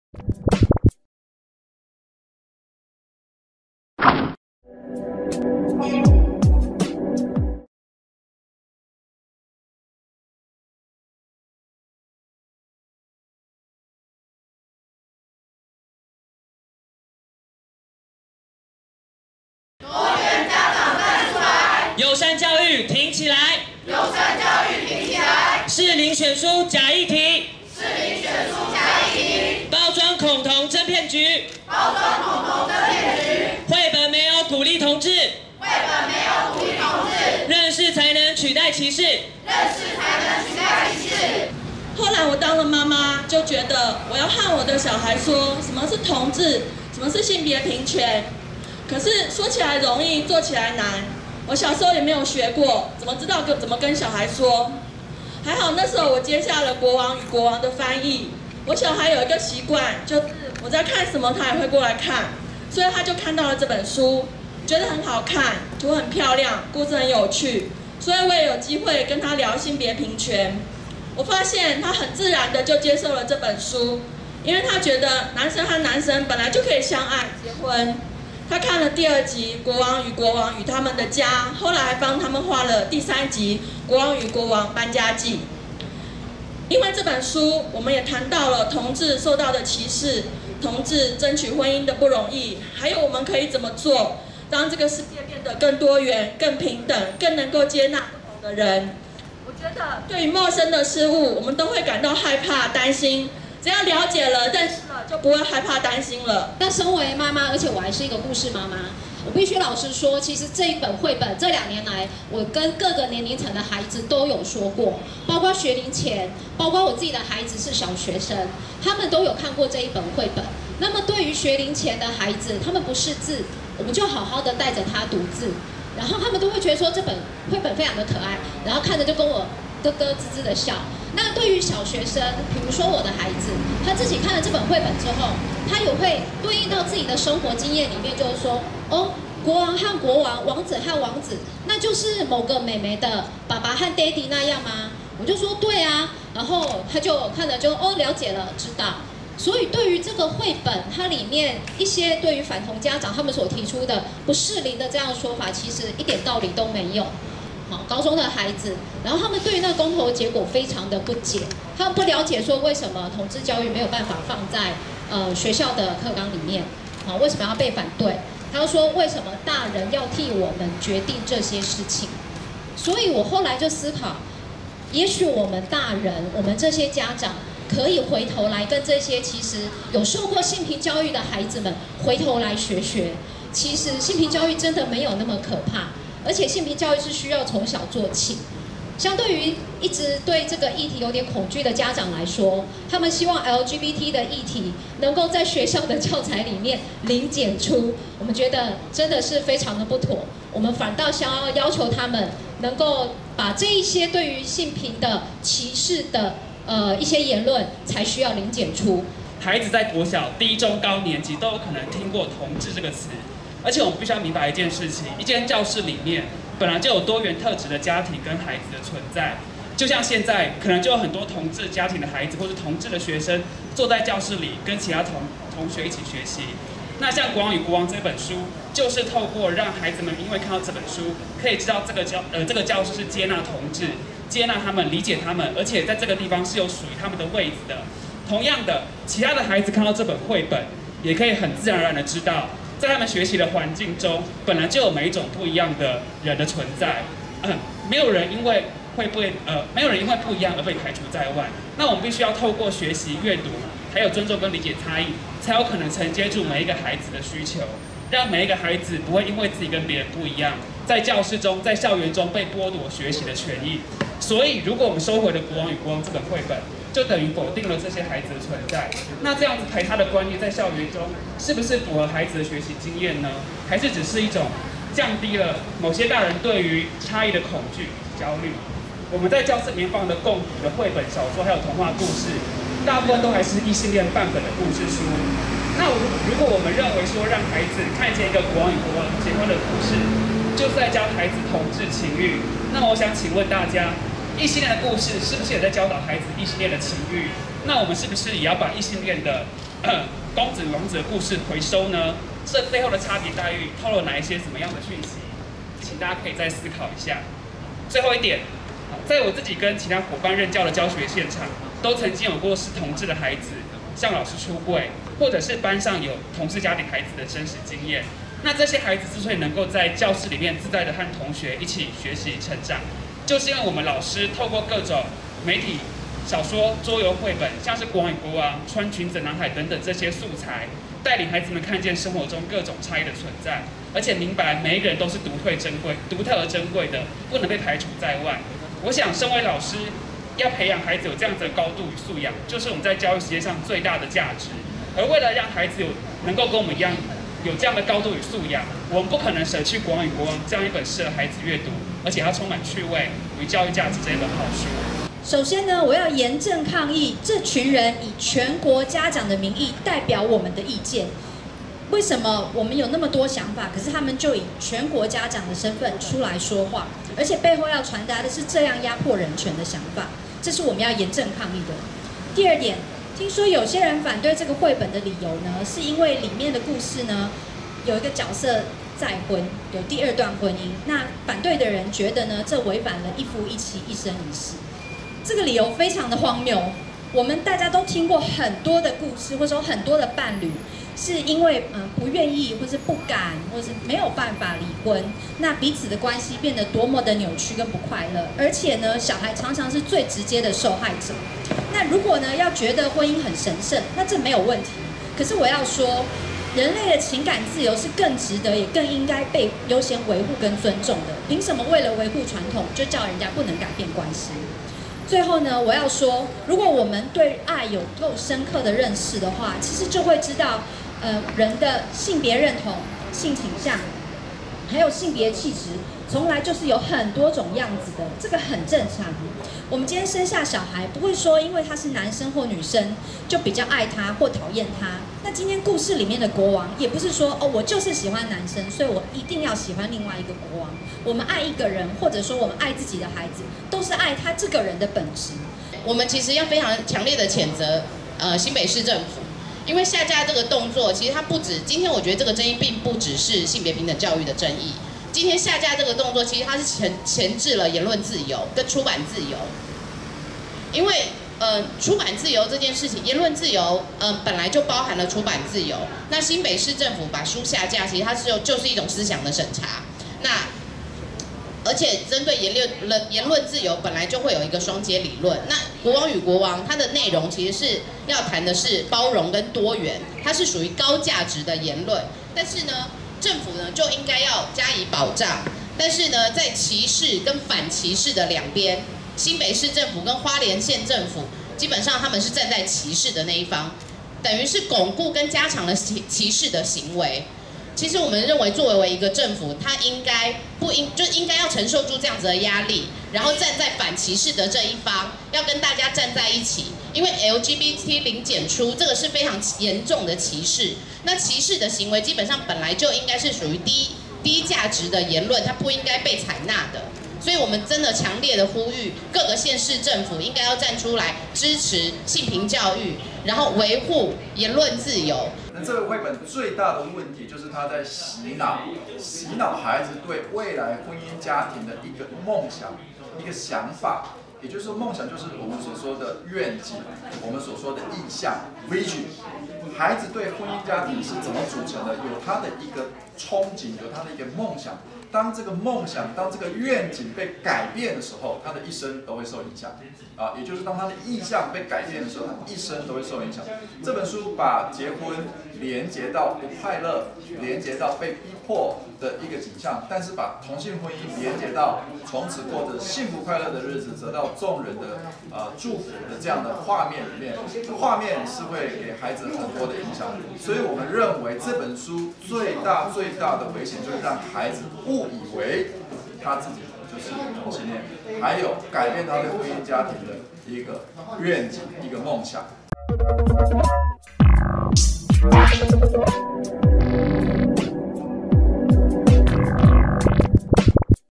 2020.09.09 教育部百大書籍《國王與國王》有爭議？ 反同團體籲「回收」、挺同團體籲尊重多元性別
日前教育部施行「109年國民中小學新生閱讀推廣計畫」，推薦100本書籍給國中及國小學童閱讀，並發送書籍給學校單位及國中小學童。其中，國小「百大好書」排行榜第60名的繪本《國王與國王》引發爭議，繪本故事情節引起反同團體與挺同團體不同聲浪。兩方團體9月9日各自召開記者會，針對教育部及該繪本表達不同立場與訴求。